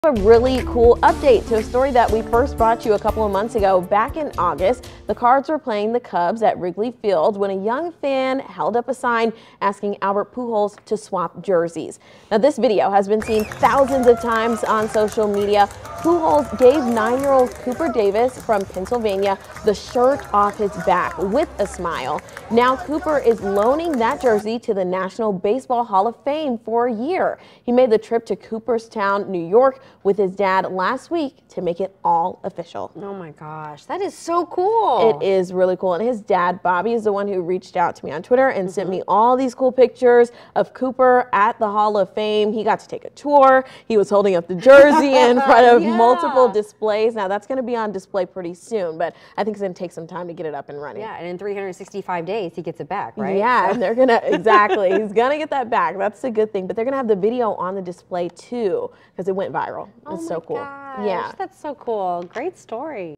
Kid loans Albert Pujols jersey to National Baseball Hall of Fame
In August, 9-year-old Cooper made a sign asking Pujols to swap jerseys, and Pujols gave him the shirt off his back.